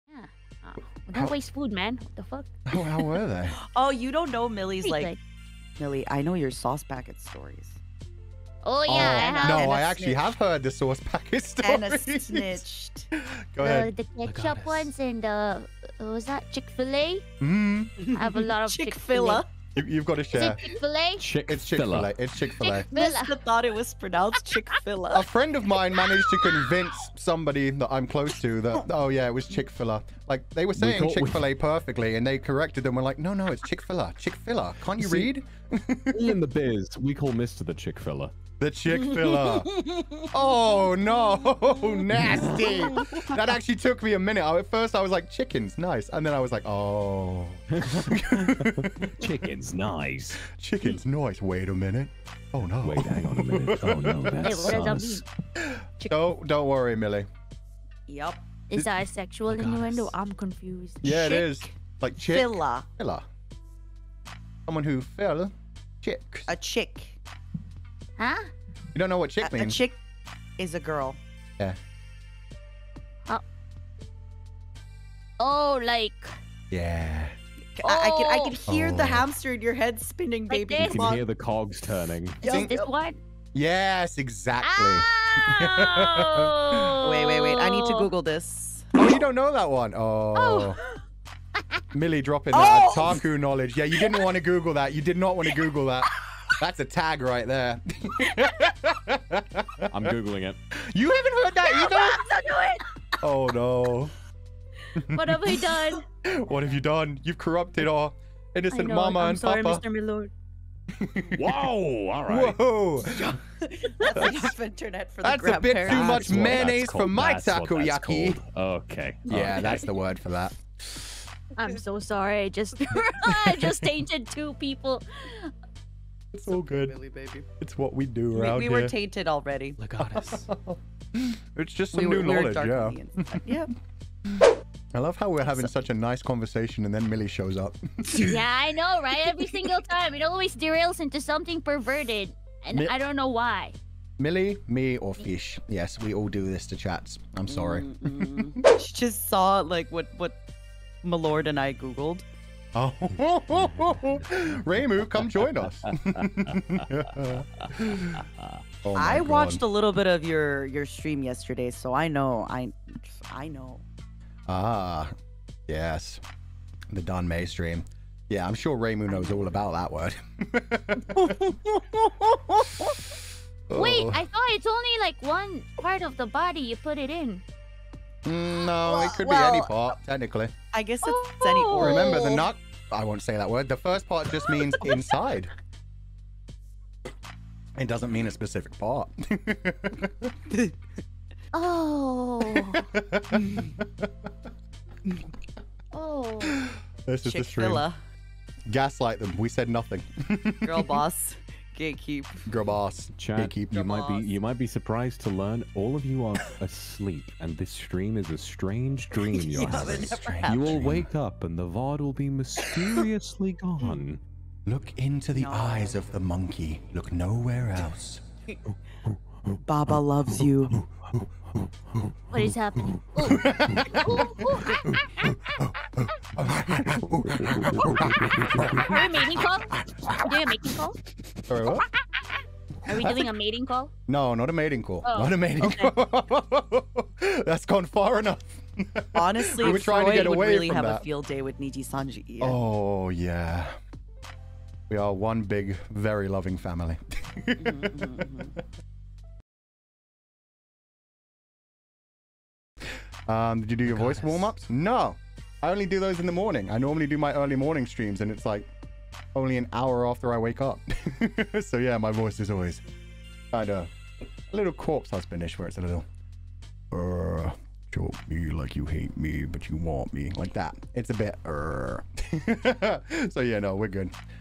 The best way to forget FORBIDDEN Yabai Knowledge! 【NIJISANJI EN】
Chaotic and unseiso conversations are the best kind of chill conversations between Nijisanji Members.